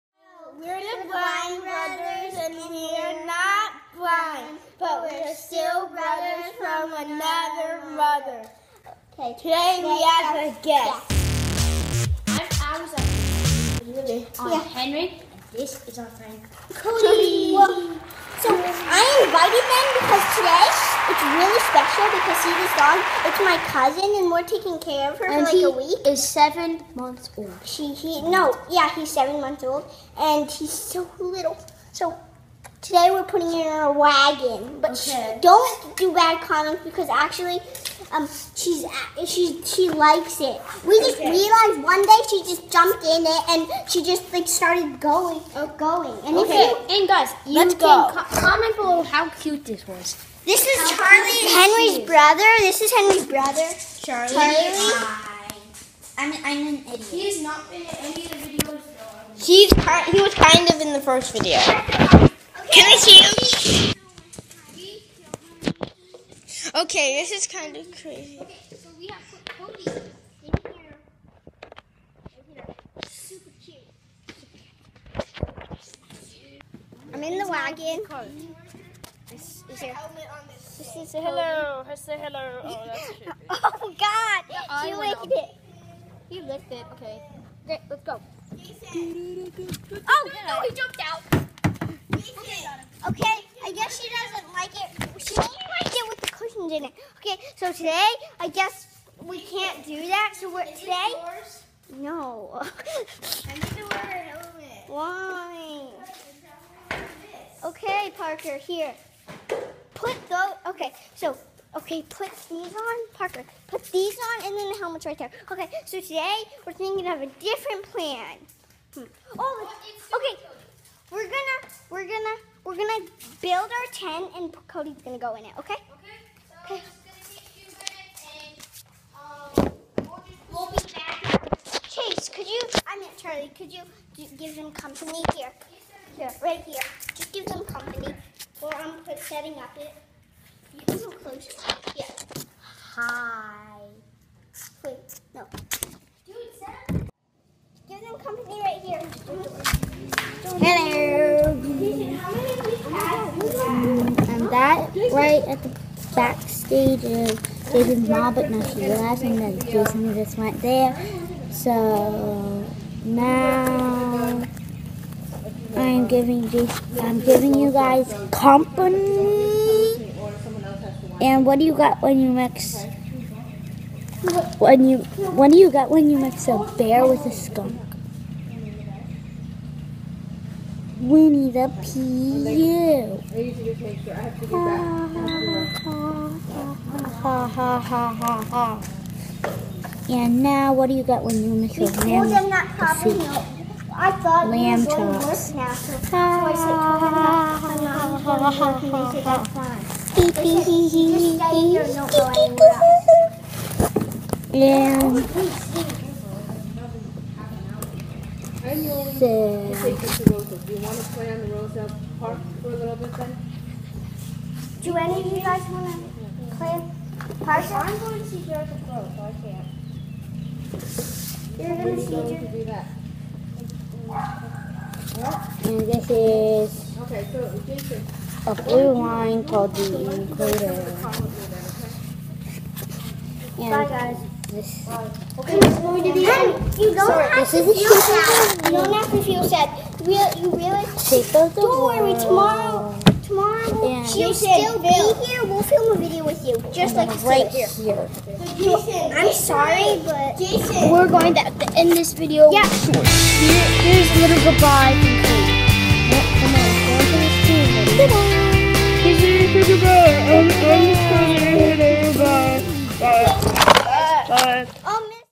Putting dogs in wagons!?
Today we played with Cody! Like this video for a video with Jason's dog!